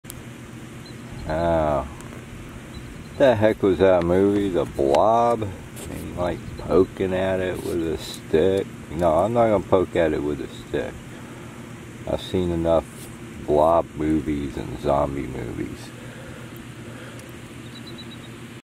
The Blob Don't Touch That
This jellyfish found itself high and dry.